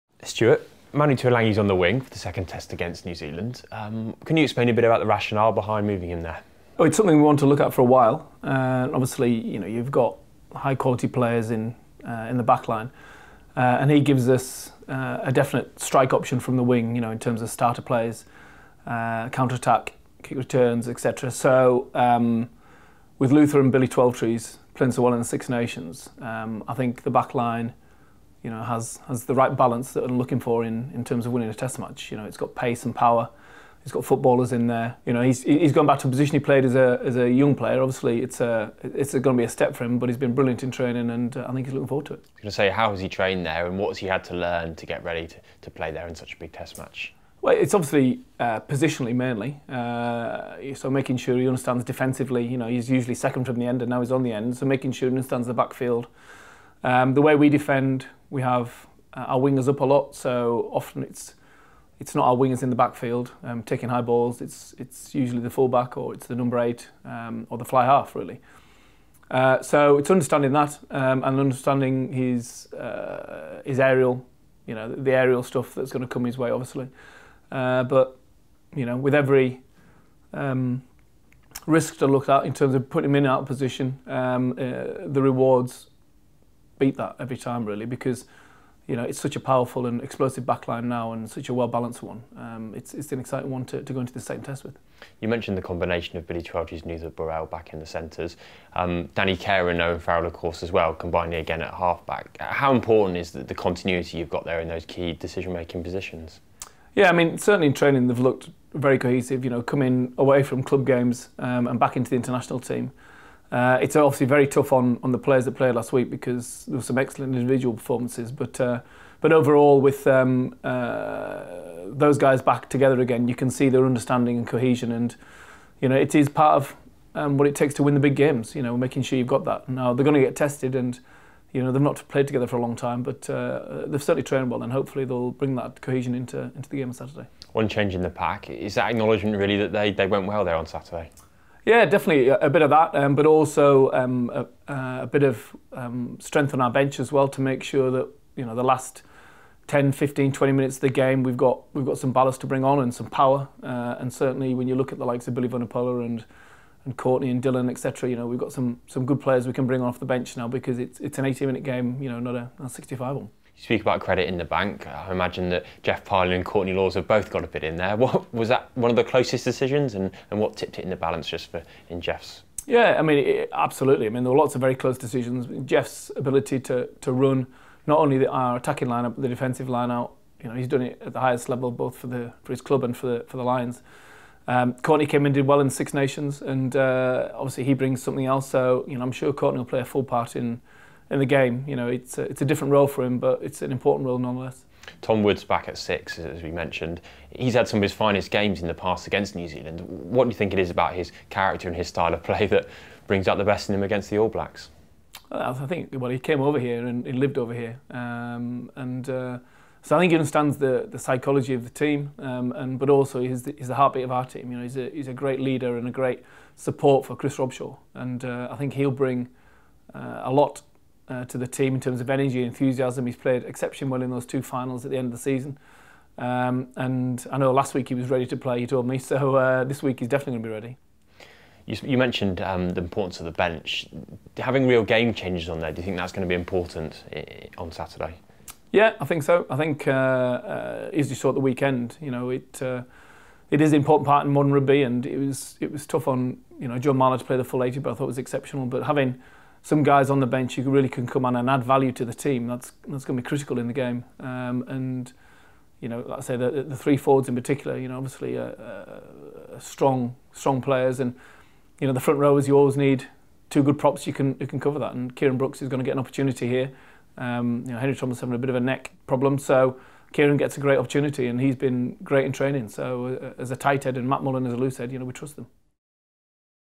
Lancaster on Tuilagi and his overhauled backline
England Head Coach Stuart Lancaster speaks to RFUtv about his team for the second Test against New Zealand in Dunedin on Saturday, with five changes and two positional